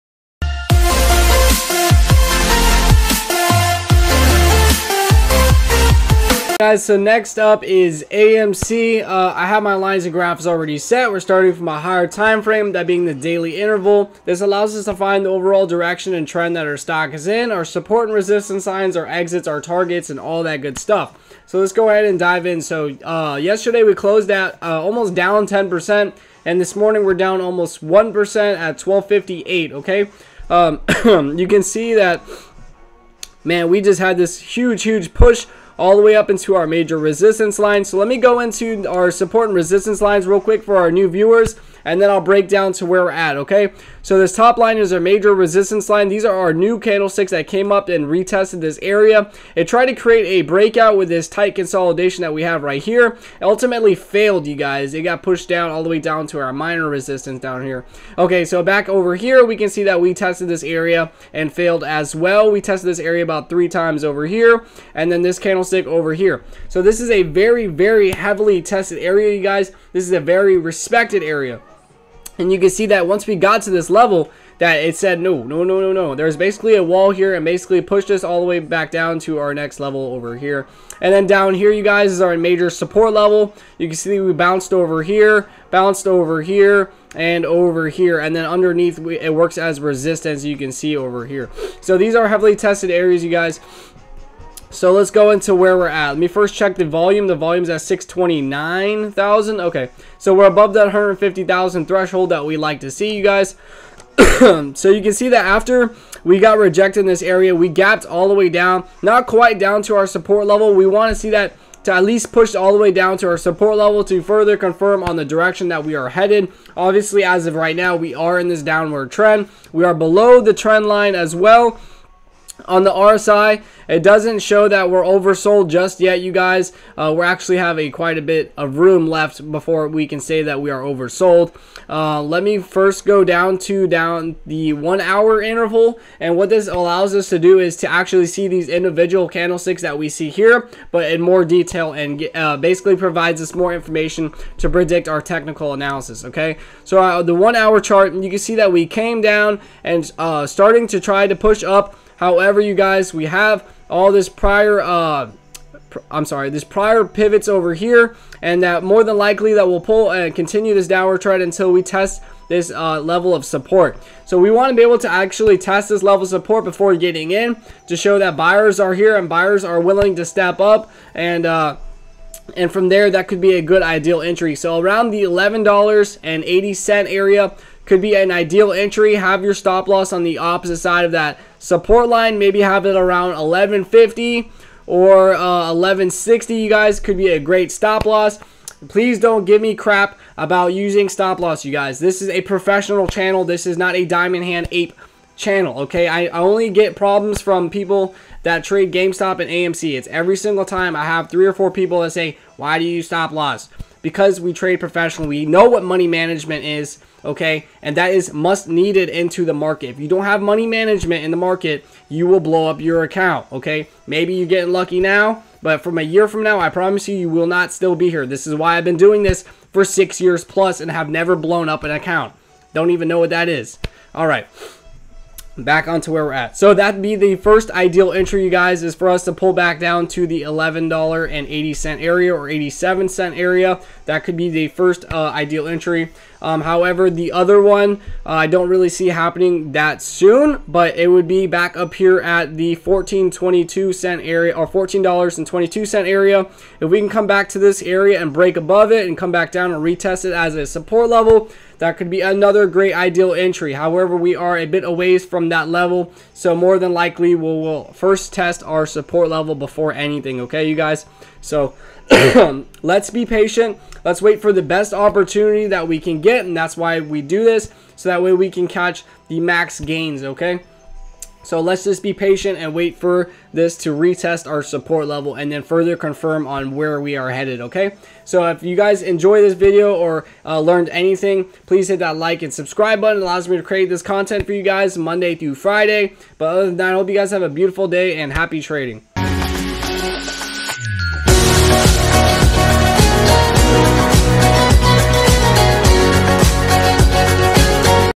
AMC STOCK PRICE PREDICTION! Amc Analysis (Update) 2021
I break down AMC using technical analysis.